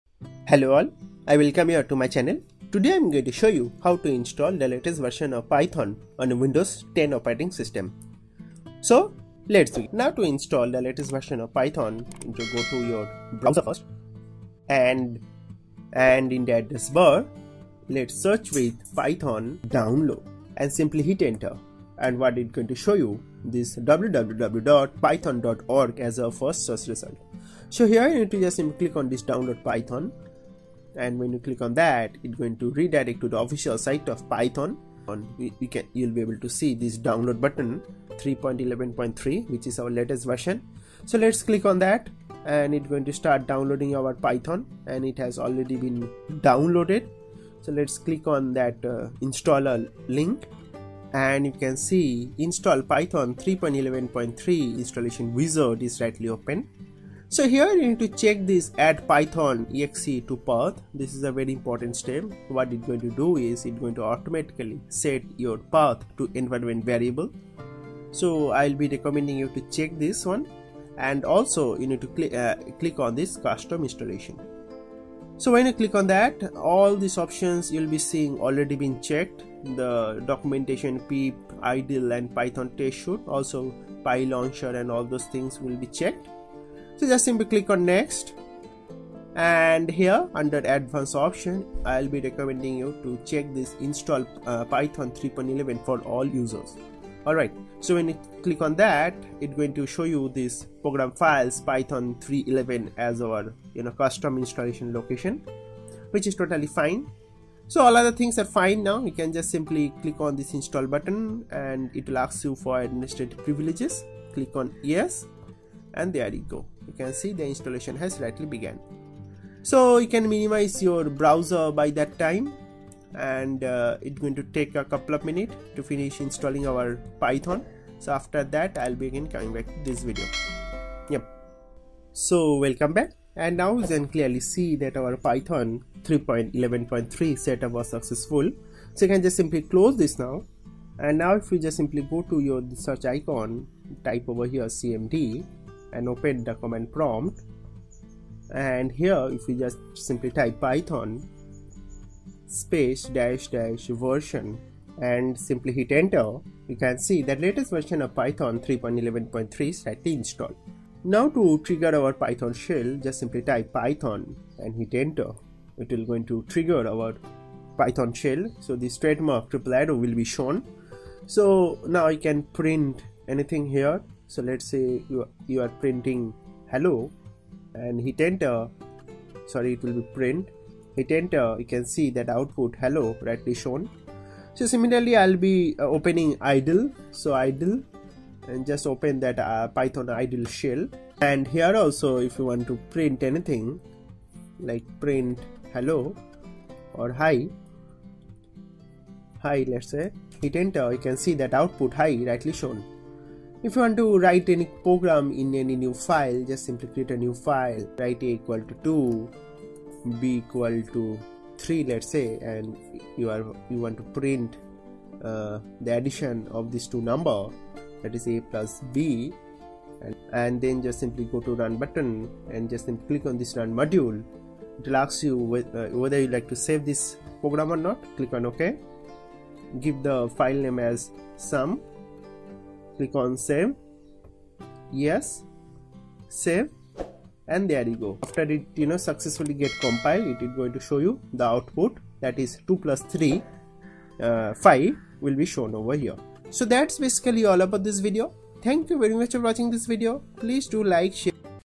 How to Install Python 3.11.3 on Windows 10 | 2023 Update
Hello All! Today in this video I am going to step-by-step guide you on How to install Python 3.11.3 on Windows 10. Windows (64-bit).

Steps to install Python 3.11.3
Step 1. How to Download Python 3.11.3?